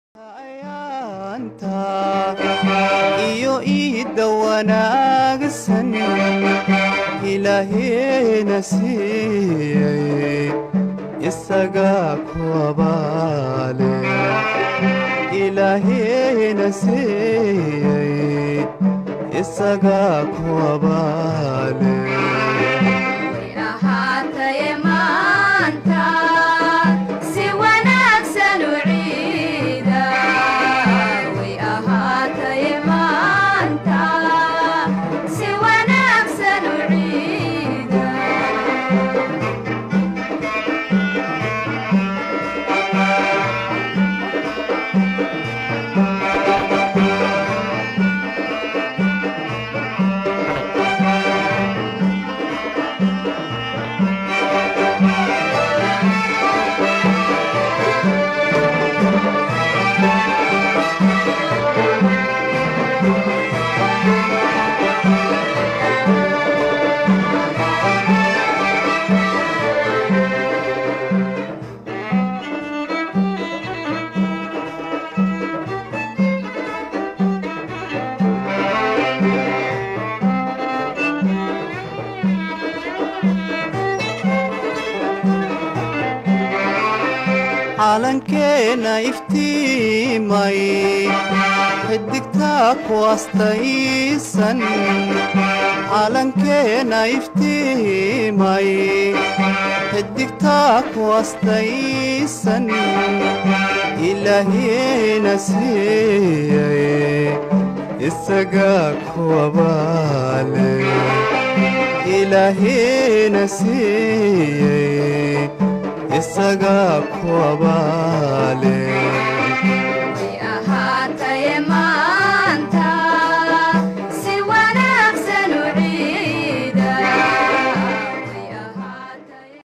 Raiisul Wasare Rooble Aya Maanta Waqti La Qaatay Shacabka Somalia Farxada Munasabada Ciidul Fatriga.
New Mogadisho City Somalia.